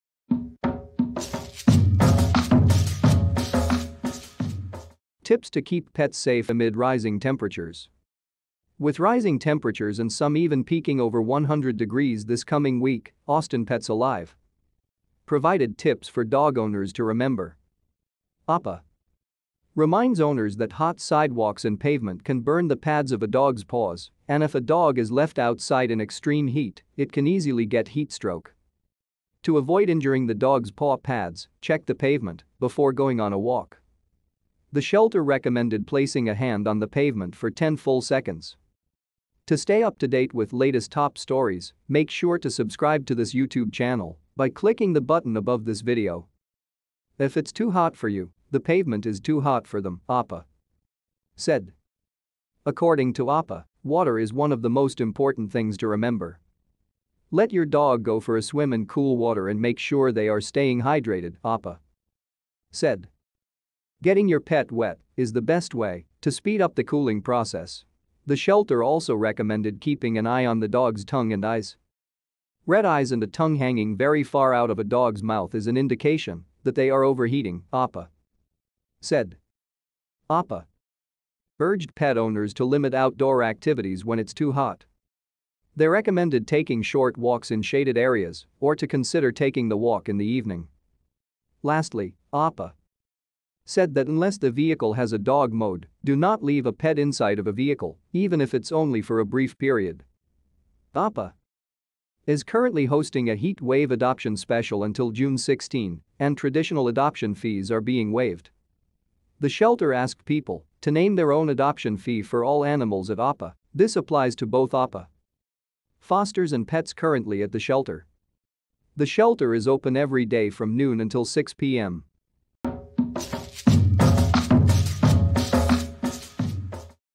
Tips to keep pets safe amid rising temperatures
With rising temperatures and some even peaking over 100 degrees this coming week,  Austin Pets Alive!
provided tips for dog owners to remember.
APA!
reminds owners that hot sidewalks and pavement can burn the pads of a dog’s paws, and if a dog is left outside in extreme heat, it can easily get heat stroke.
To avoid injuring the dog’s paw pads, check the pavement before going on a walk.
The shelter recommended placing a hand on the pavement for 10 full seconds.
“If it’s too hot for you, the pavement is too hot for them,” APA!
said.
According to APA!, water is one of the most important things to remember.
“Let your dog go for a swim in cool water and make sure they are staying hydrated,” APA!
said.
“Getting your pet wet is the best way to speed up the cooling process.”

The shelter also recommended keeping an eye on the dog’s tongue and eyes.